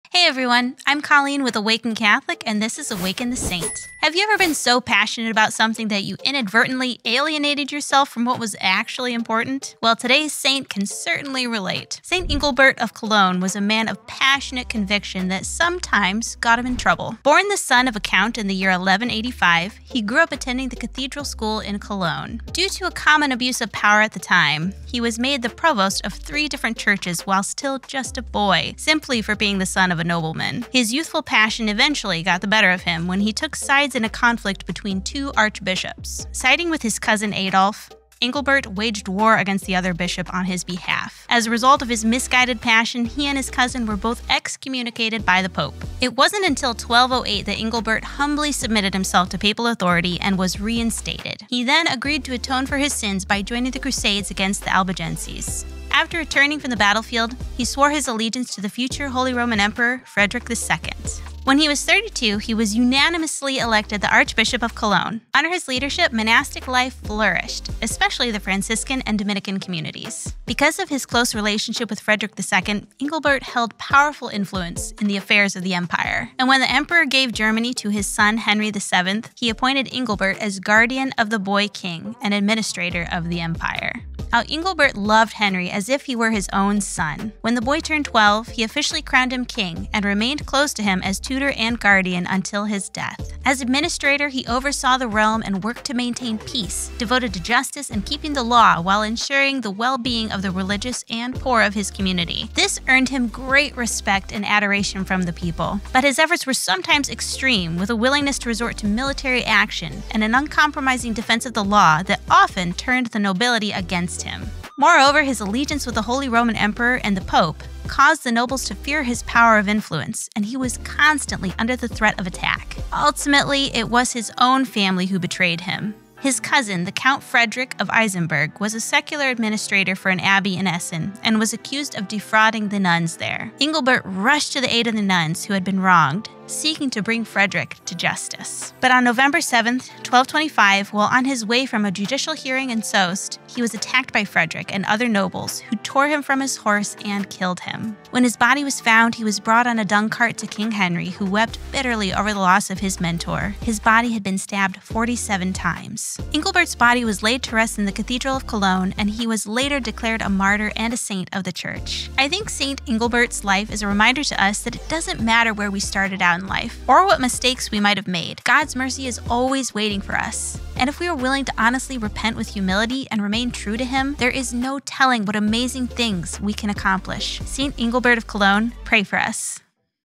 St. Engelbert of Cologne: Passion, Power, and Justice
Always one to take a passionate stance for justice, Engelbert held great influence upon the kingdom of Germany.

Saint Engelbert of Cologne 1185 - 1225. Feast day: November 7.

Show Host: Colleen Manahan